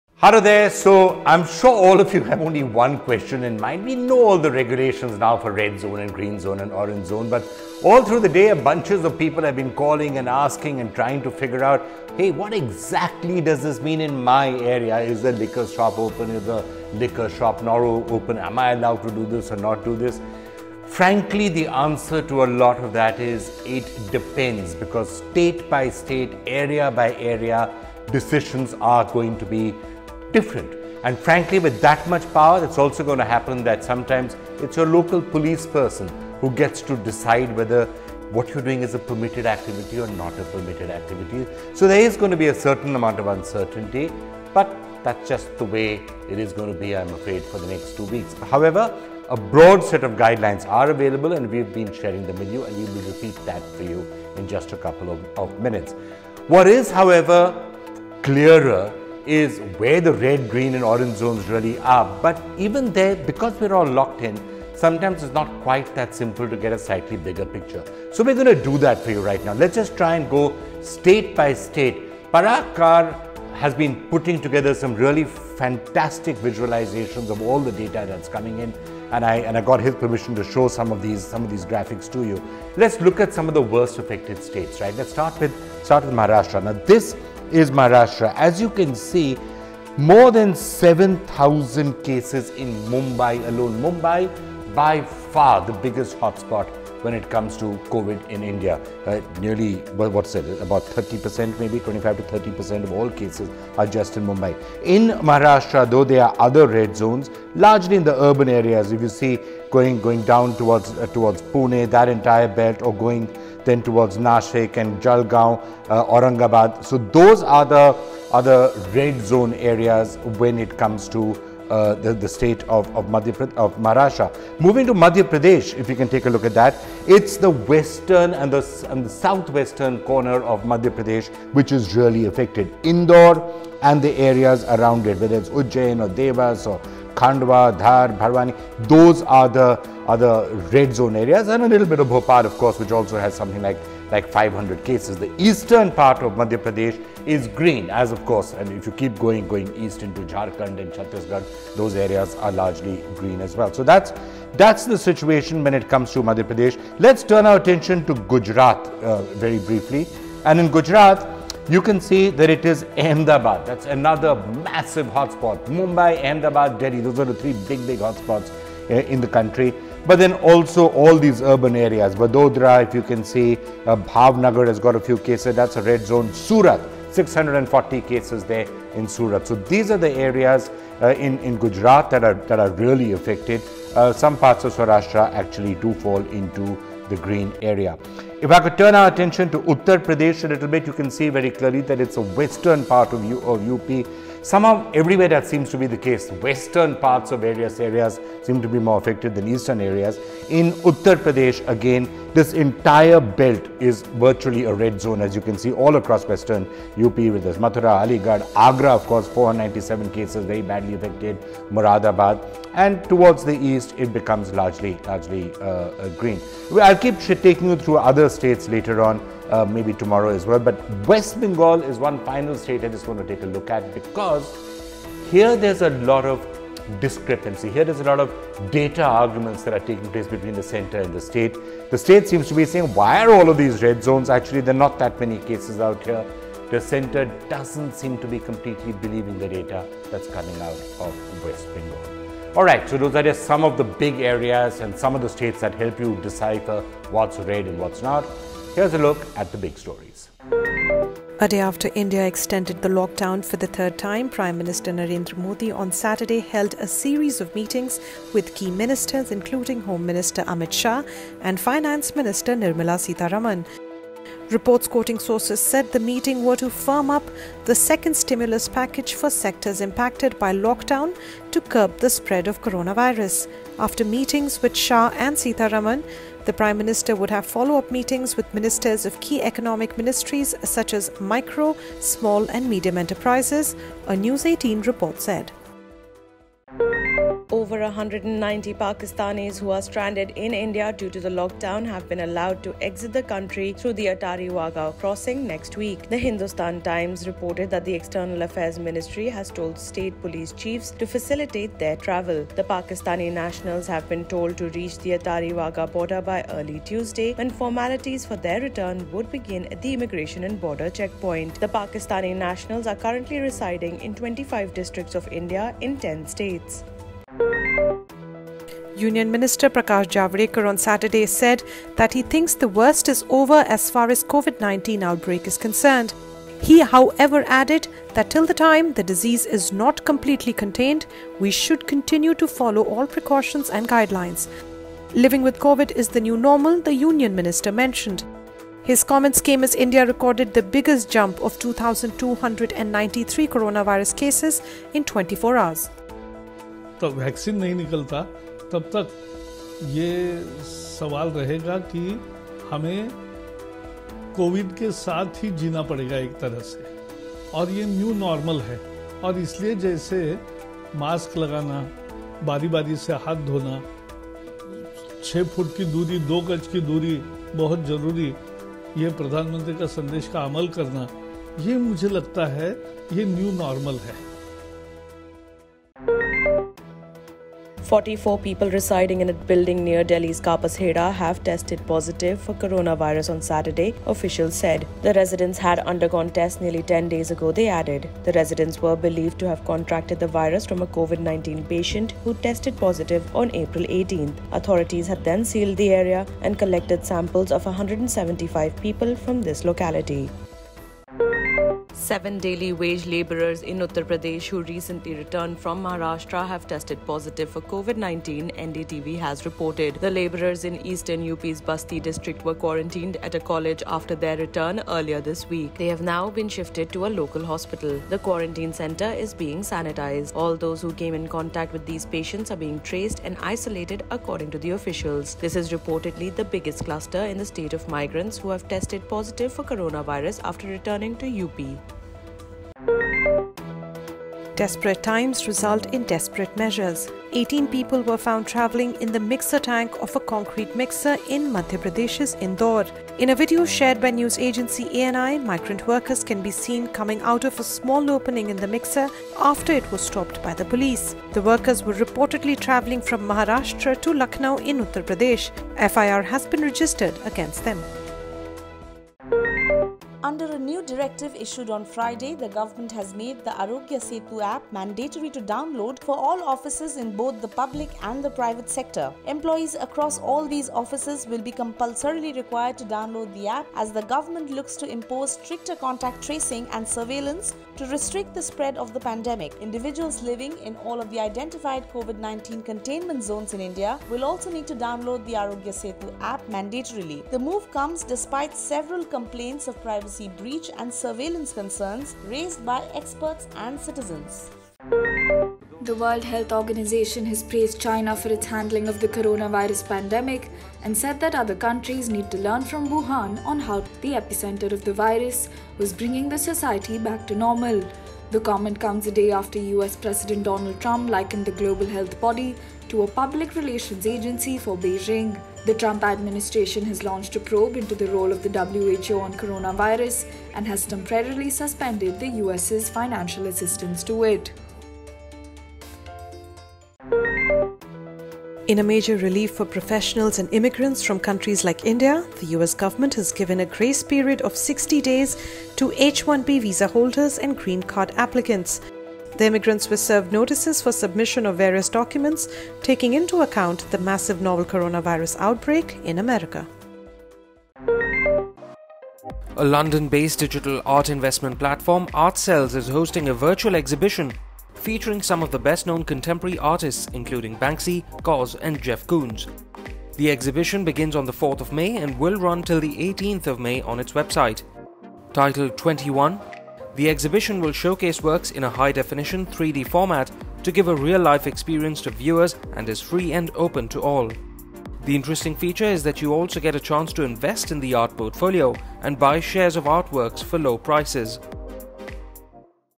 Covid-19: An in-depth analysis of hotspots with Vikram Chandra
The Ministry of Home Affairs on Saturday issued a new notification, clarifying the passenger rules in the Orange Zone. In this episode, Vikram Chandra explains why a lack of clarity on the lockdown rules may hinder India’s fight against Covid-19 and follows it up with an in-depth analysis of the virus hotspots across the country.